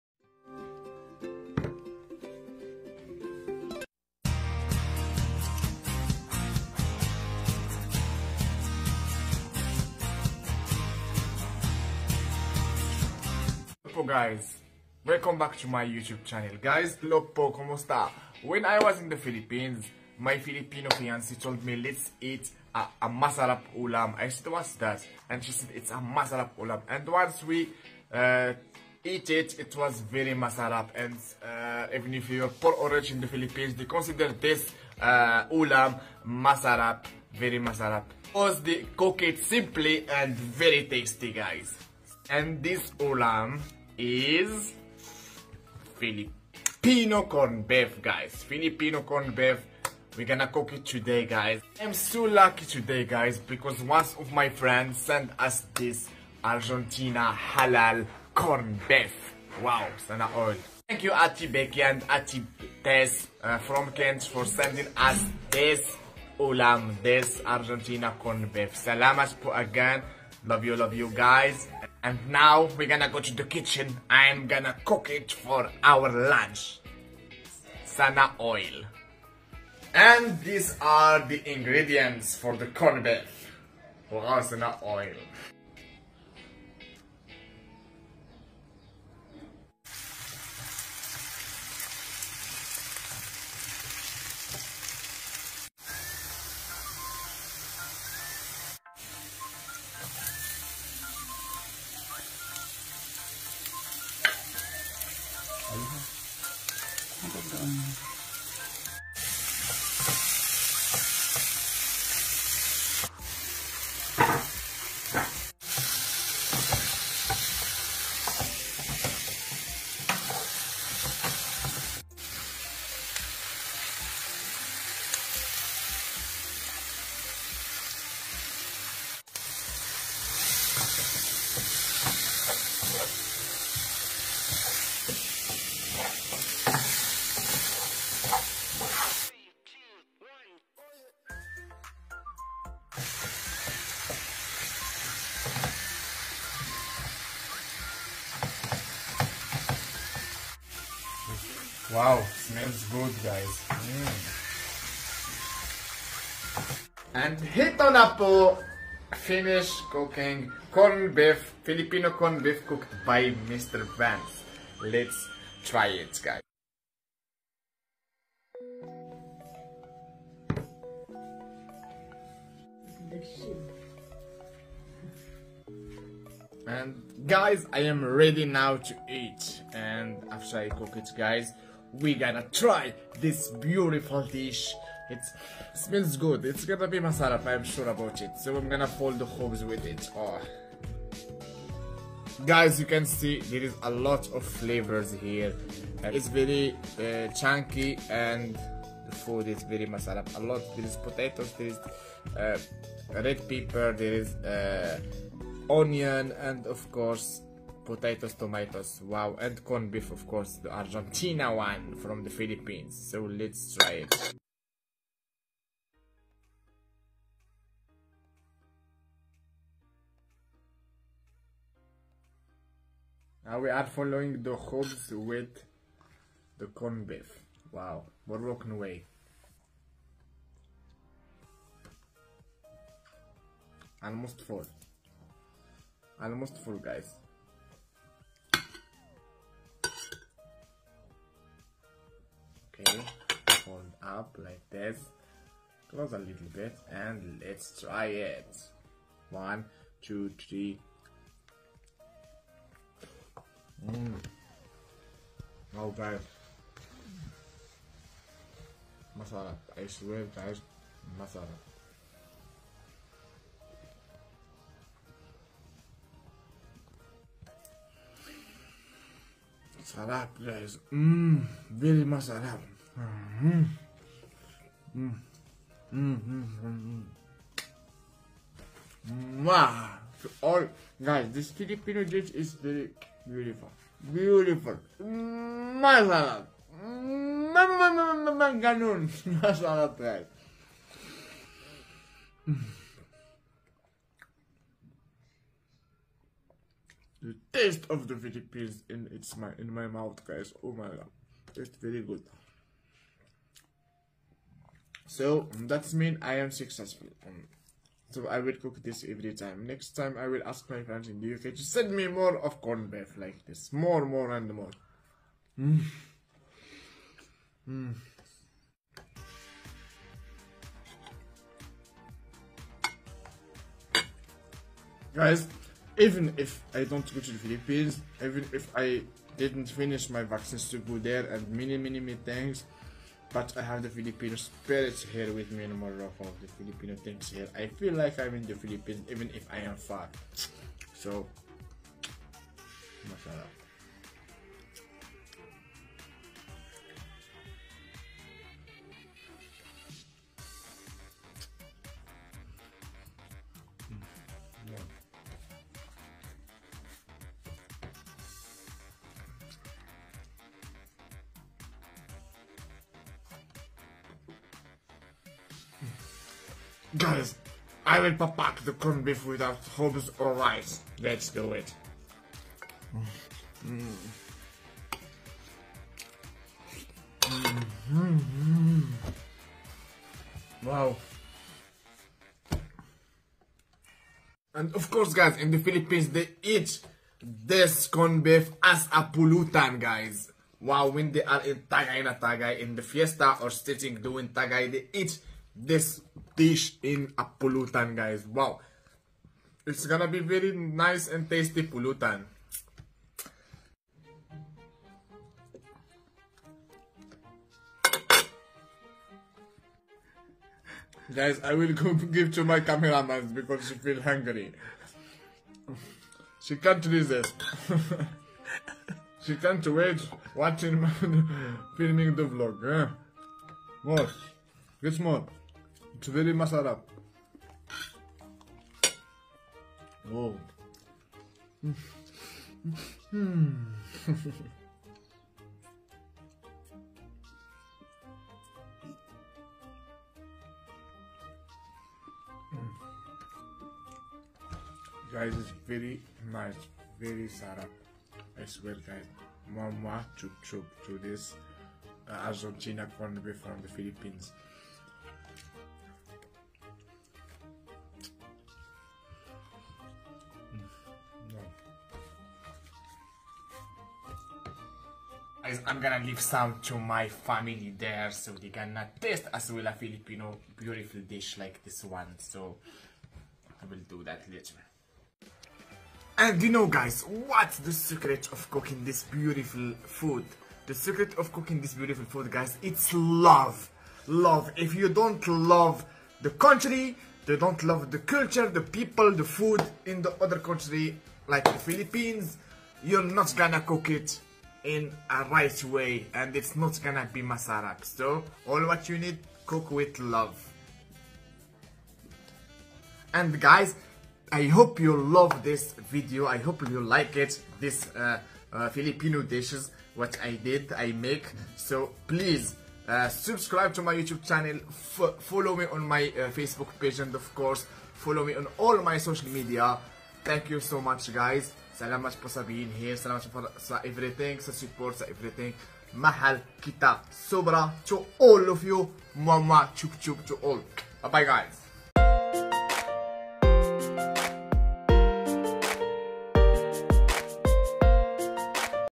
Filipino 🇵🇭 Corned Beef Light Mukbang in Morocco 🇲🇦 Cooked By Mr.Vanz ♡
I cooked Filipino Corned Beef in Morocco for the first time and it was very masarap , after that I did à light mukbang for you guys .